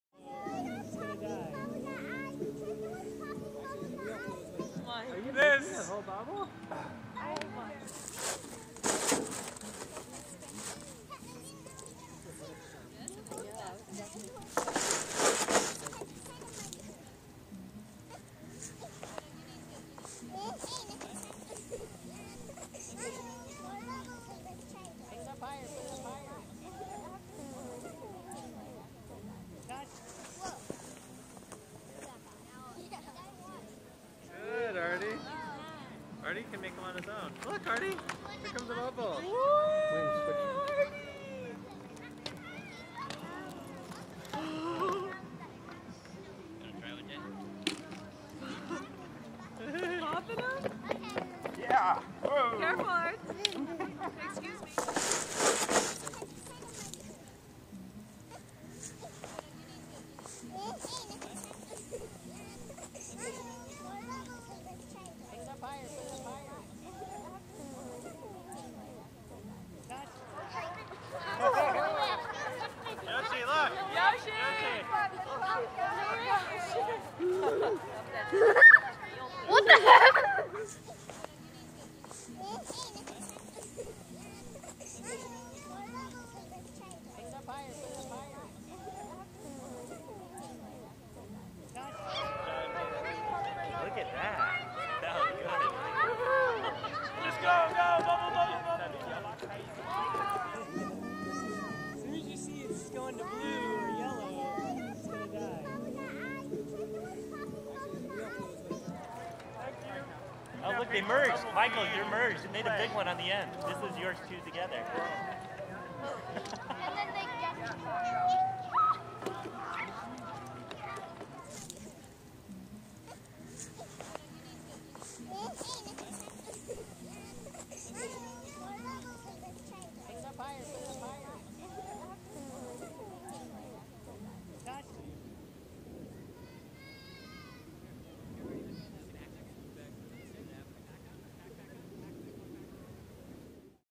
Giant Bubble Party IV
Serious Bubble Quantity and Quality!
December 17, 2011
Saratoga, CA (Foothill Elementary School)
With Sterling Johnson and Brian Lawrence